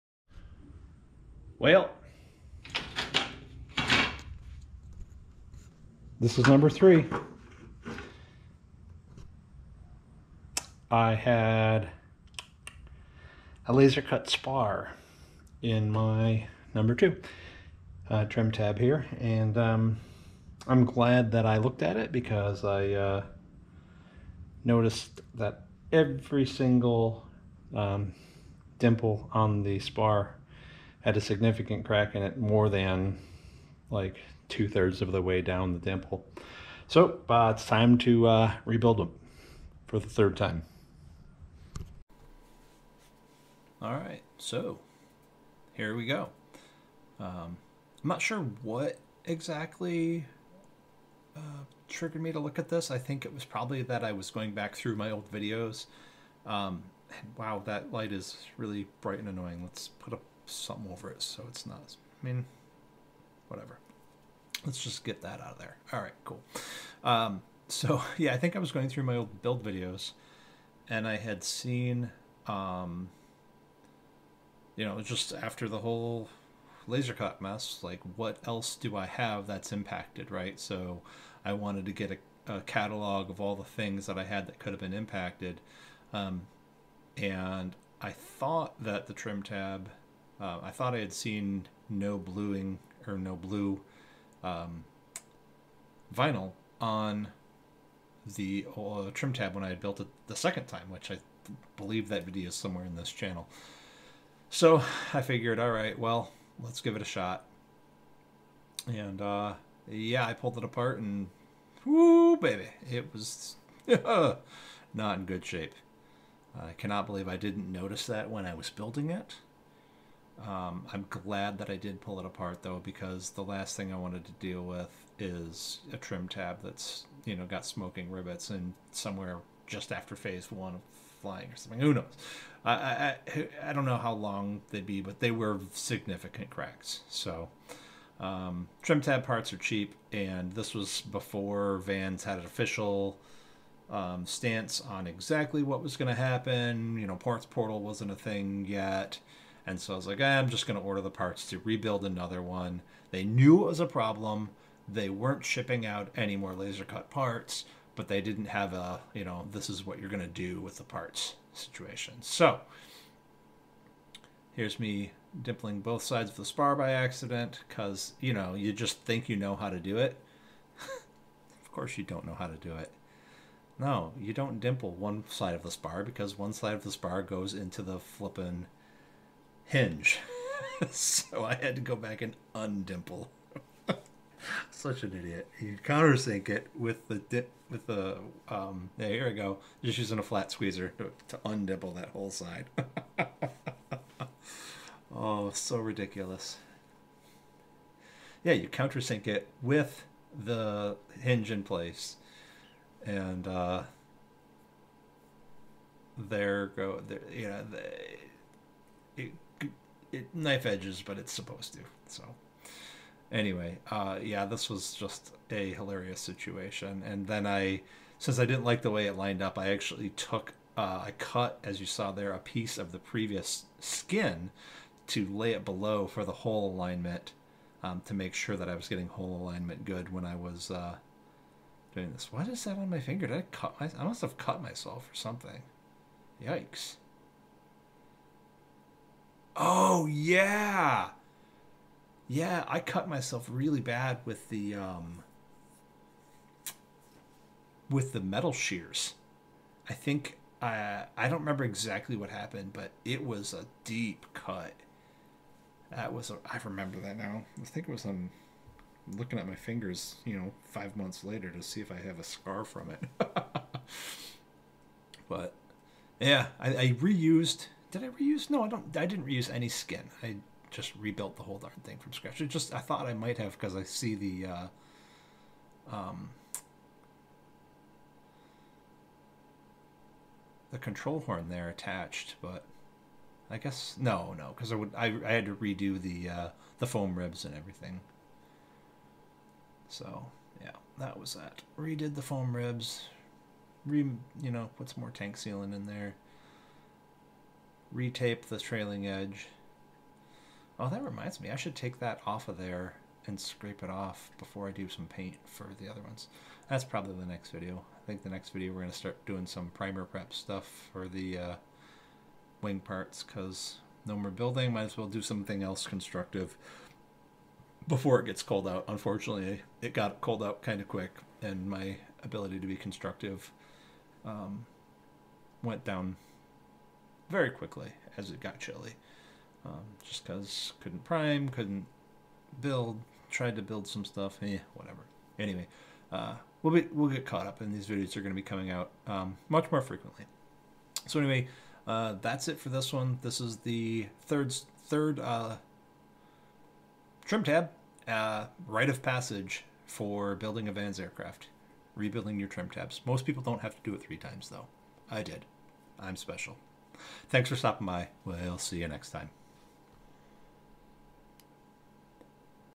Third time's a charm... Trim tab edition!
When you've got a laser cut spar with cracks in your trim tab.. you rebuild it.